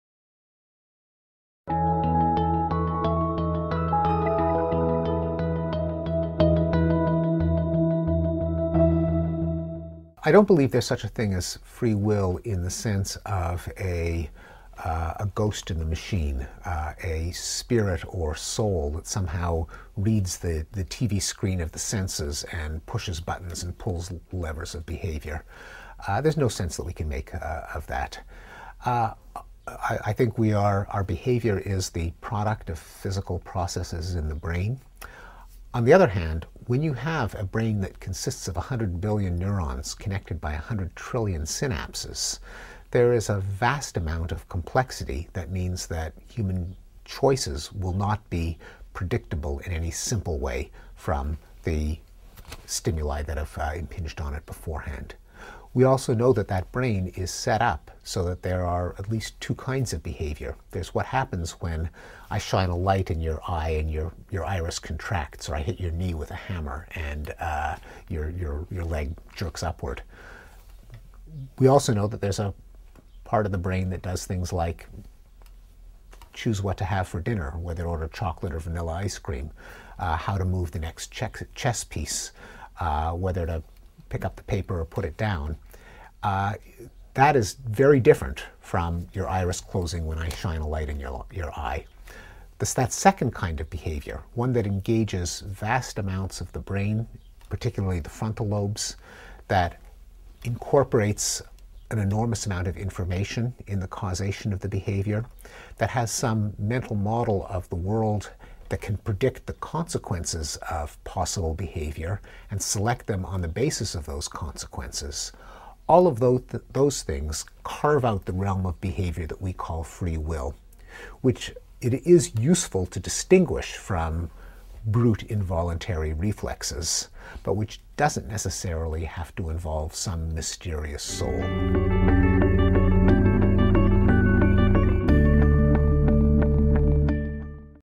Steven Pinker sobre el Libre Albedrío (Subtitulado)
Video propiedad de Bigthink

Alma máter:
1 - Dawson College,
2 - McGill University,
3 - Harvard University

Se especializa en:
psicología evolutiva, psicología experimental, ciencia cognitiva, lingüística y cognición visual